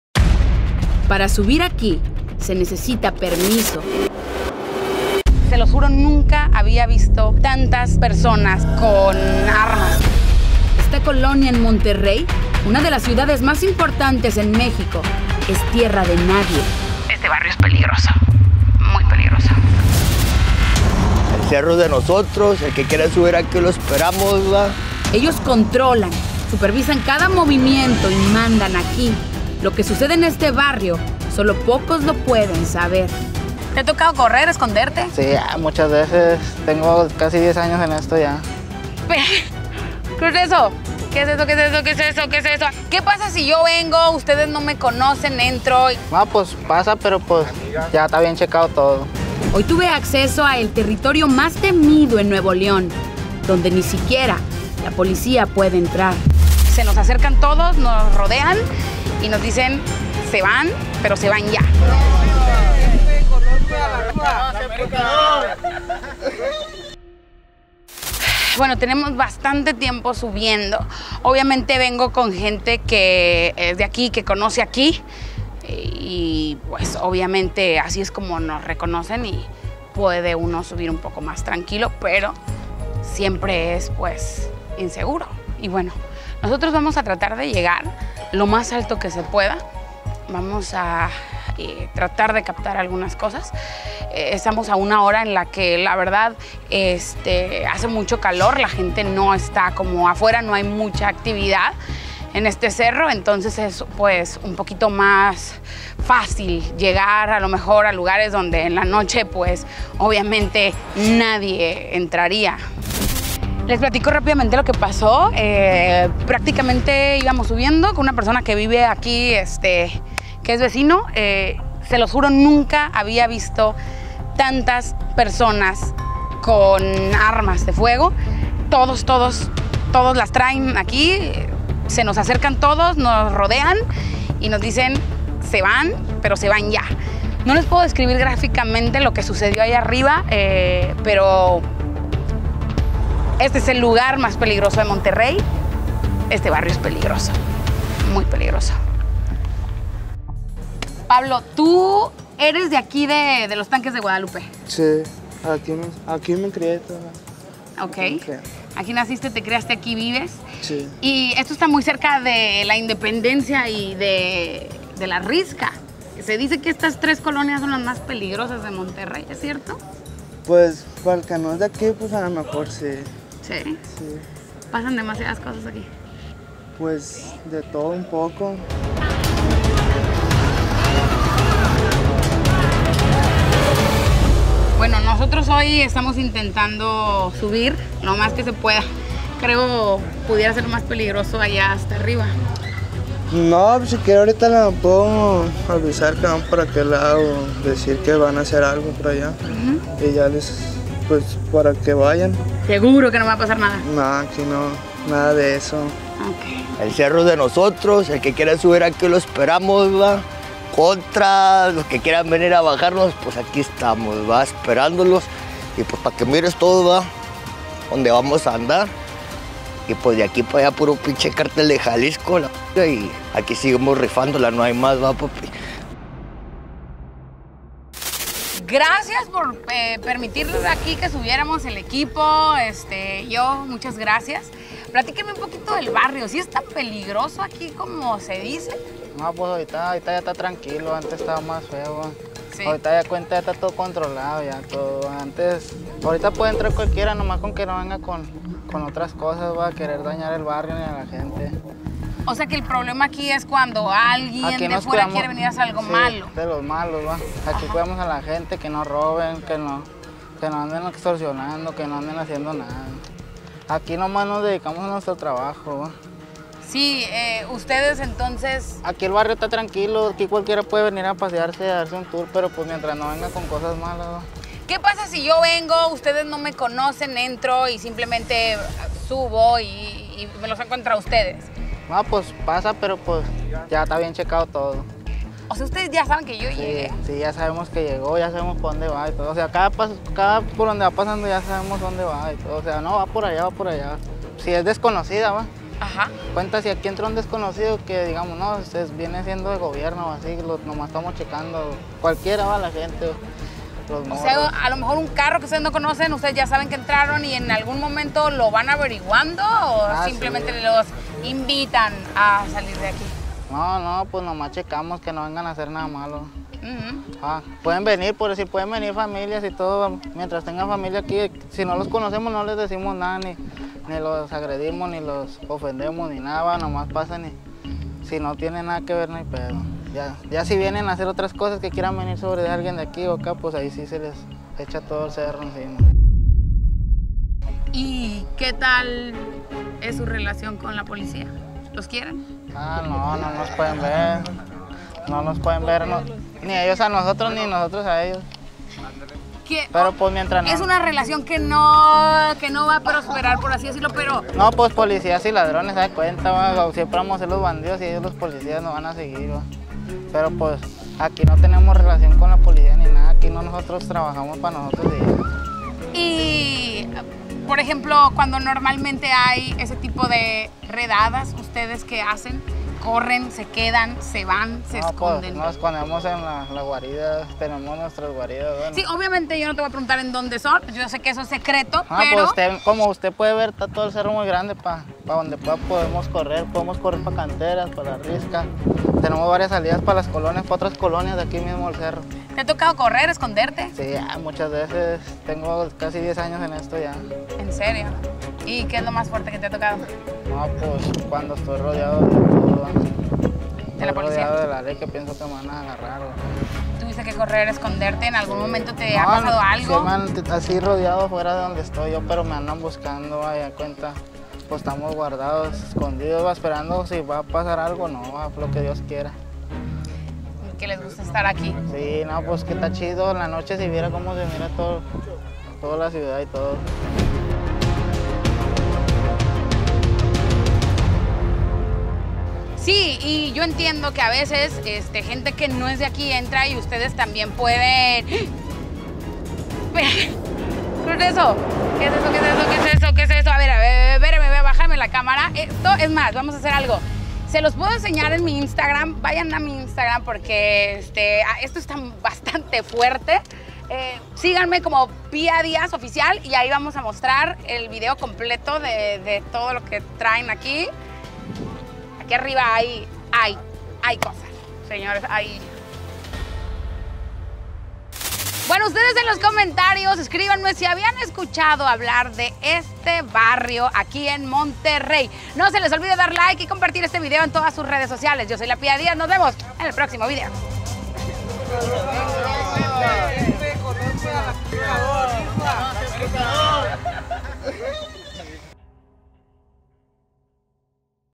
Visité la colonia más peligrosa de Monterrey
Para subir aquí se necesita permiso, esta colonia en Monterrey, una de las ciudades más importantes en México es tierra de nadie. @LaPiaDiaz nos lleva en un recorrido por uno de los barrios de más cuidado.

BADABUN NETWORK ©